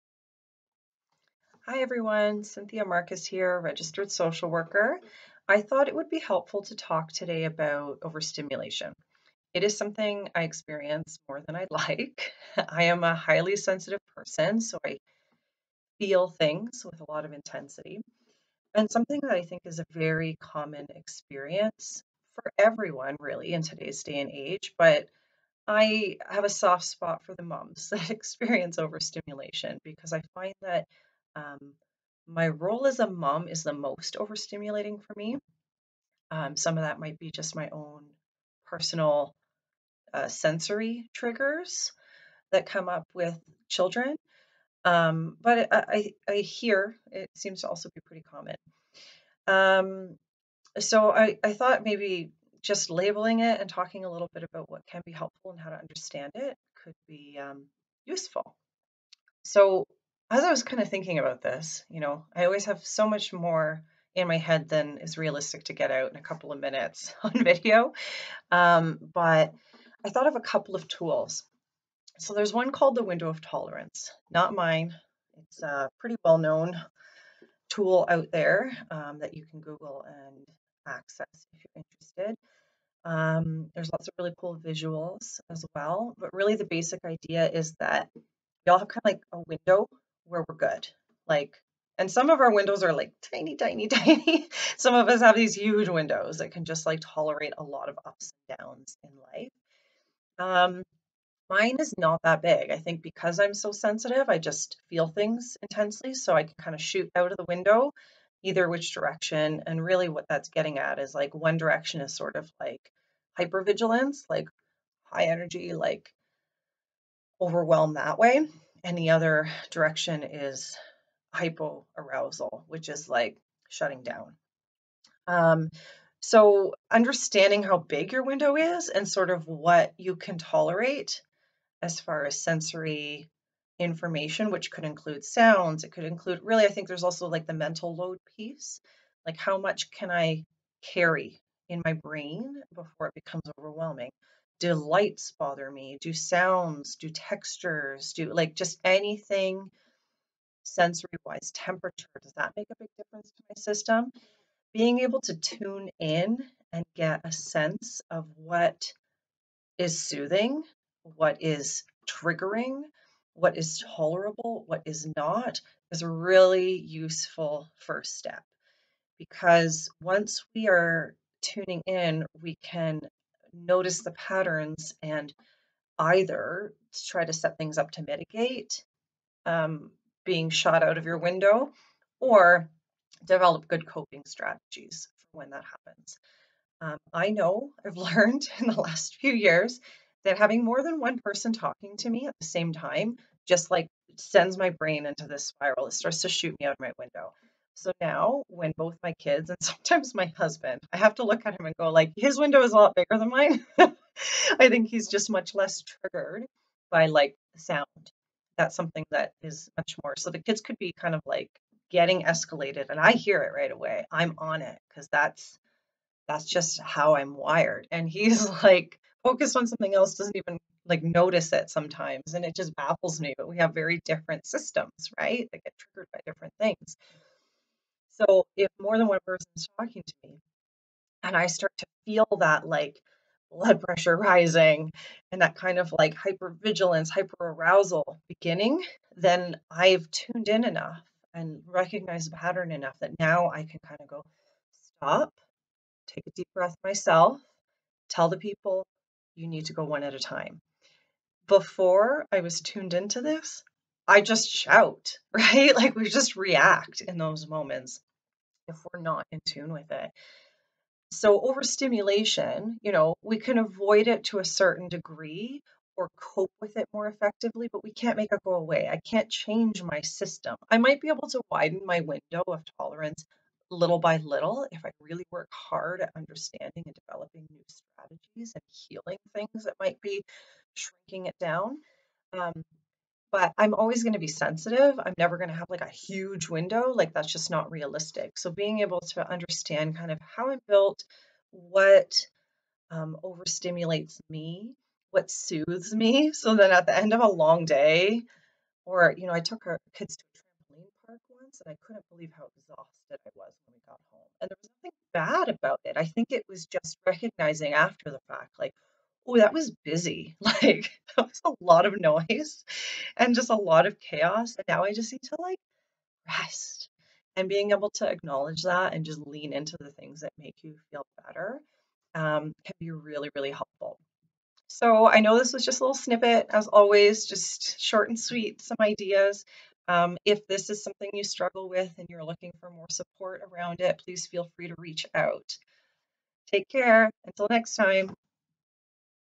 Overstimulation
It is so common for people in today's world to feel overstimulated. We're often multi-tasking, with a huge to-do list on our minds, and sensory information like sights and sounds being thrown at us from every direction. While overstimulation is common among most people, I think overstimulation in motherhood is even more common. The endless noise, needs, and lacking boundaries with small kids can be a lot to manage. In this video I talk a bit about my experience with overstimulation and how understanding our window of tolerance can help us to cope.